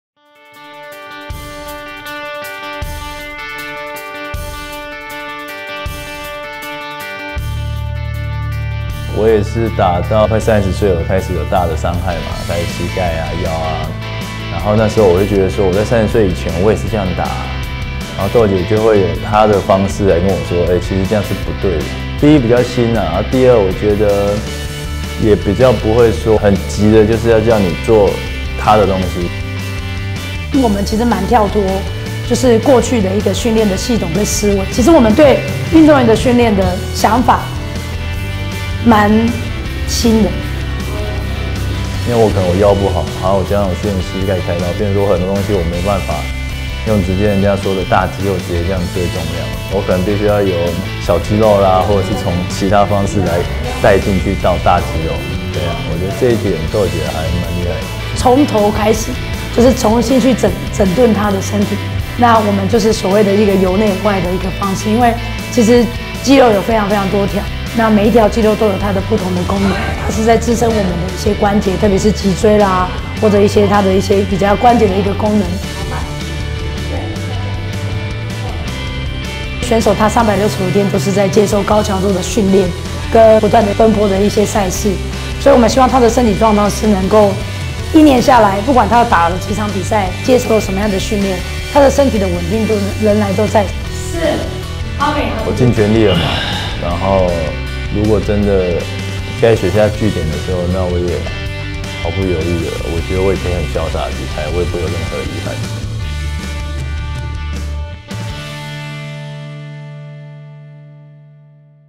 陳順詳恢復傷勢之路
因傷纏身的陳順詳
透過Deon教練的訓練方式，迅速恢復
達到生涯4000分的里程碑！